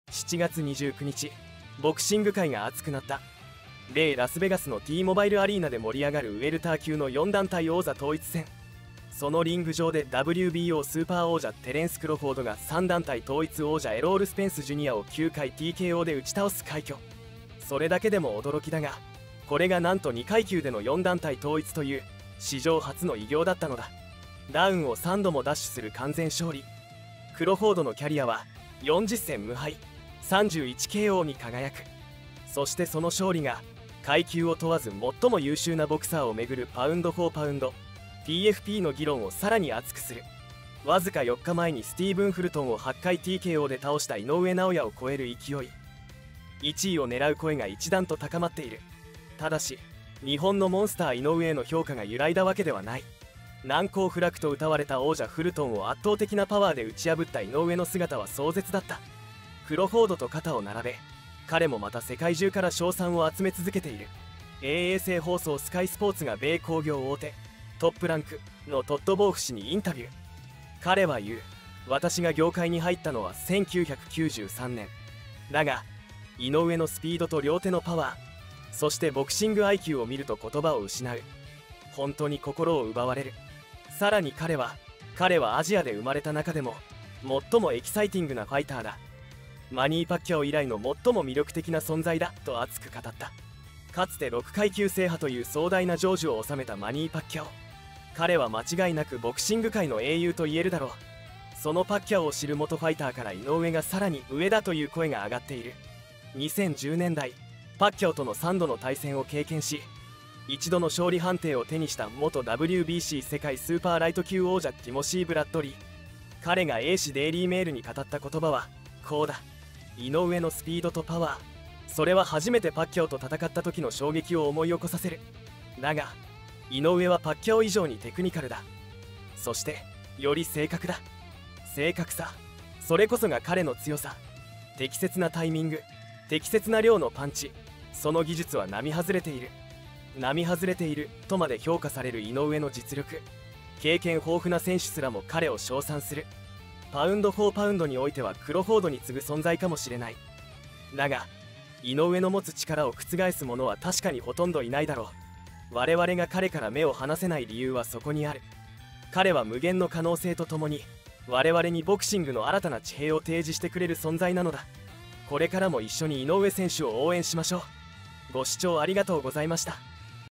パッキャオに勝った選手「井上尚弥はパッキャオより凄い。パッキャオよりも〇〇〇〇〇が格段に上！」と称賛される！【海外の反応/ボクシング】
00:00　クロフォードと井上尚弥
01:03　海外の反応
01:40　ティモシー・ブラッドリーの評価
02:20　エンディング
貴重なお時間を使っていただきありがとうございます。
井上選手はPFPでも上位ですしパッキャオ選手と比べられるのもとてもすごい！
主に格闘技の速報を頑張って編集しております！